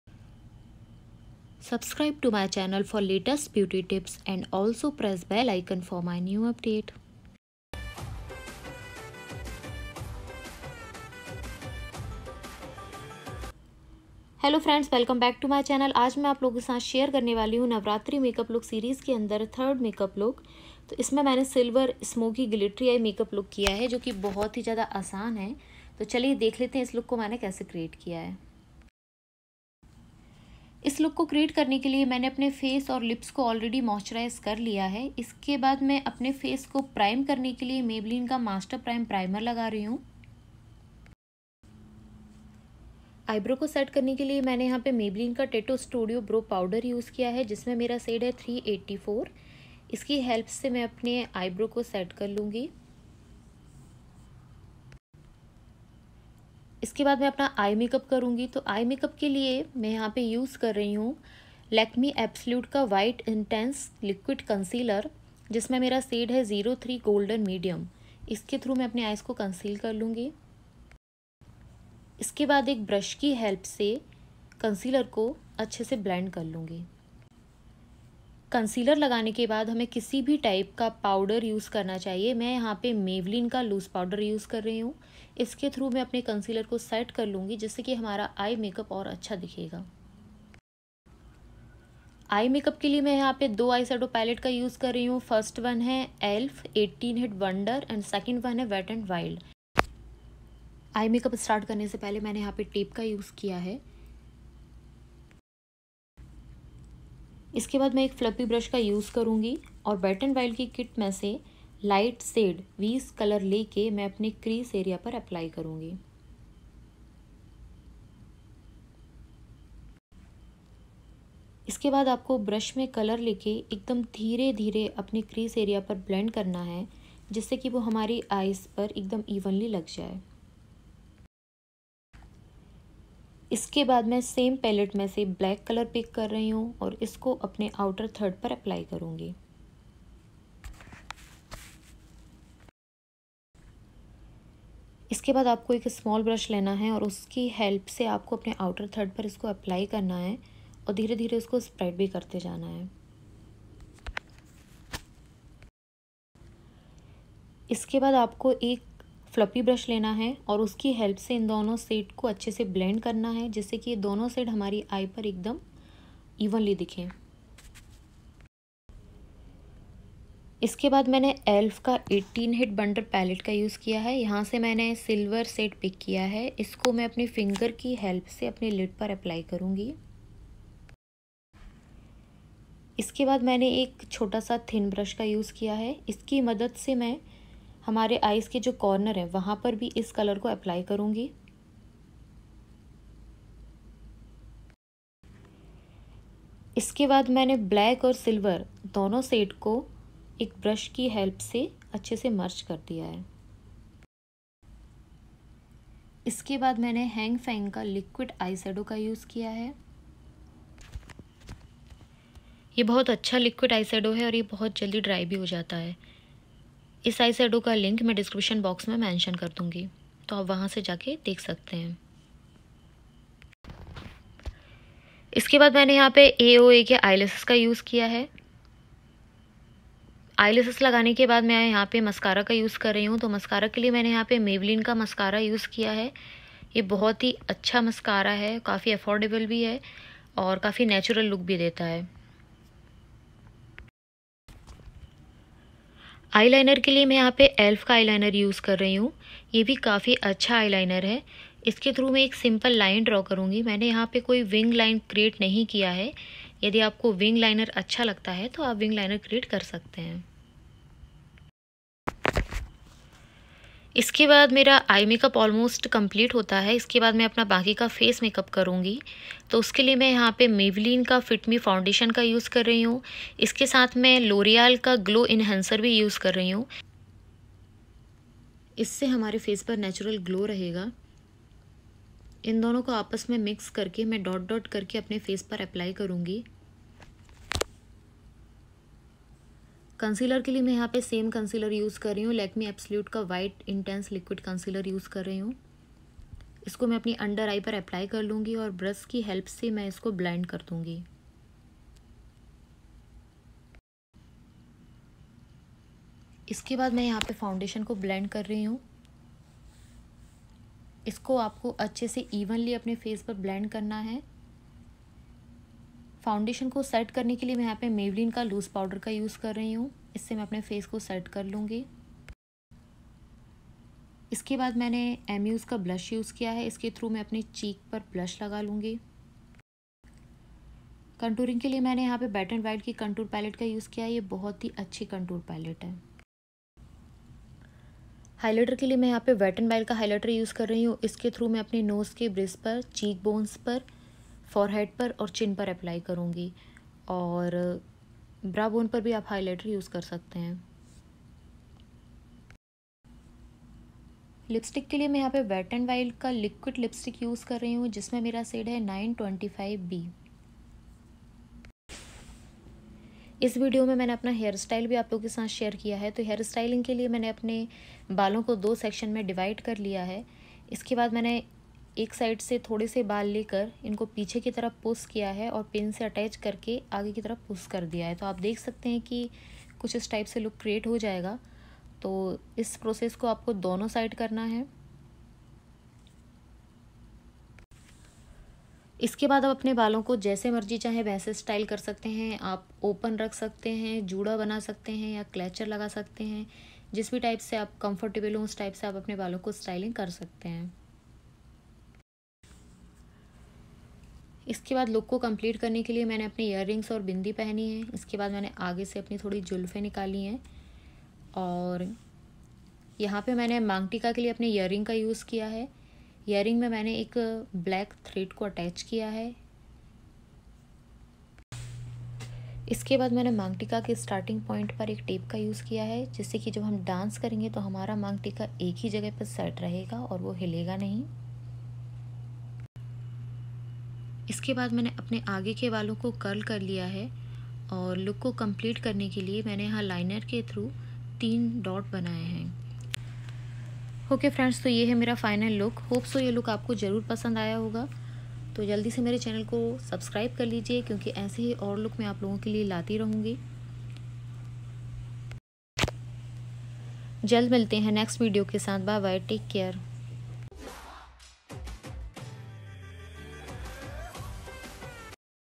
Navratri makeup look series look3rd# , durga pooja , festive makeup Tutorial ||sonam soni
Hyy girls...
Today i am sharing with you navratri makeup look. I have shared step by step makeup process so that you can also try out.  This is great idea for navratri .

Do try this makeup look and share your pics with me on instagram I would love to watching them.

Product list💁‍♀️
Primer- maybelline master prime 050
eyebrow pencil-maybelline
concealer-lekme 03 golden medium
eyeshadow pallet-elf 18 hit wonder, wet & wild
Liquid eyeliner- Heng feng silver
eye liner-elf eye liner
mascara -maybelline colossal volum
foundation-maybelline +loreal glow enhencer
blush-amuse
highlighter-wet & wild
lipistik-wet & wild 926B
setting spray-wet & wild